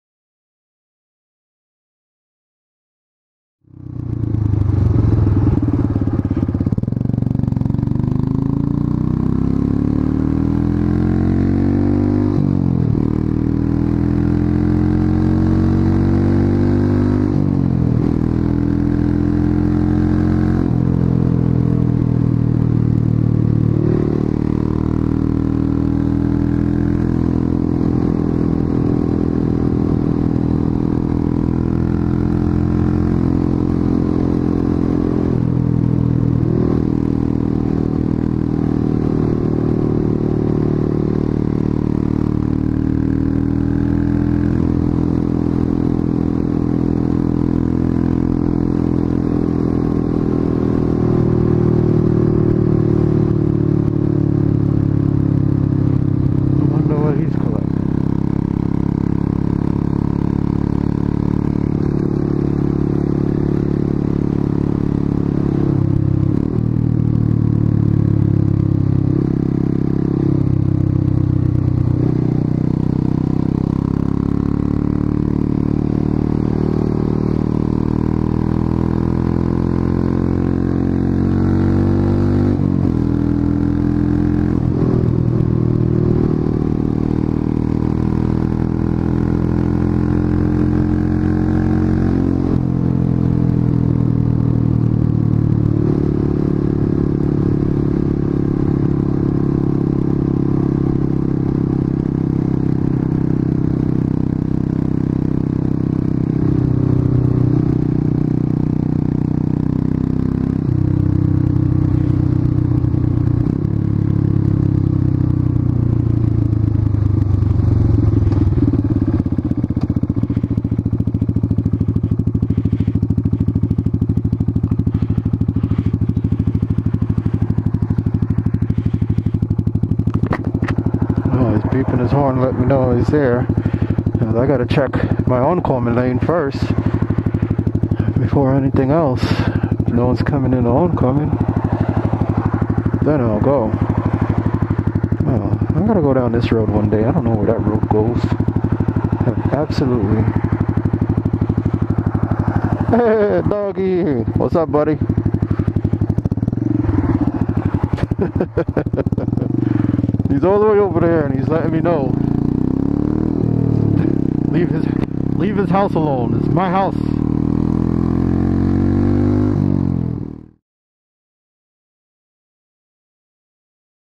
fun roads .... follow me
While Out riding, I jump onto one of my usual roads.. I am familiar with this route, and having a little fun ..
1080p 60fps(?)
Mounted on Chin of helmet